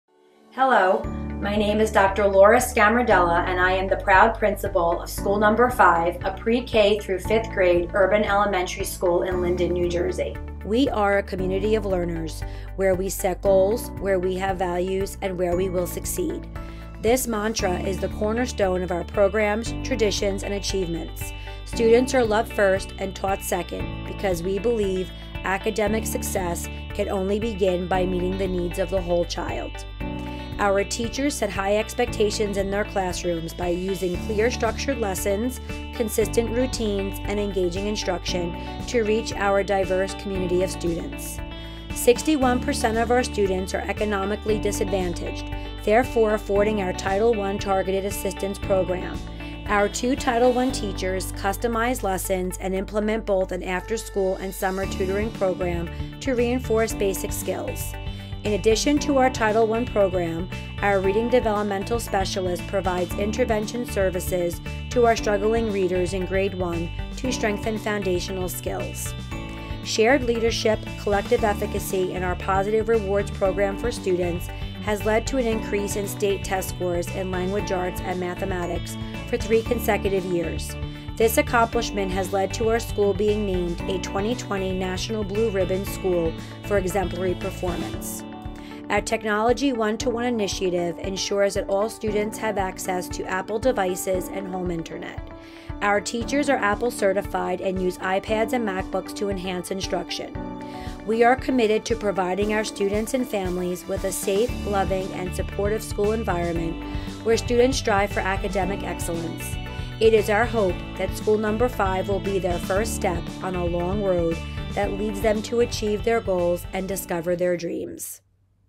School No. 5 ESEA Distinguished School
School No. 5 has been selected as a National ESEA Distinguished School by the New Jersey Department of Education in recognition of the success of its education programs and progress of its students. This video was submitted as part of that celebration, highlighting some of the things that make School No. 5 special.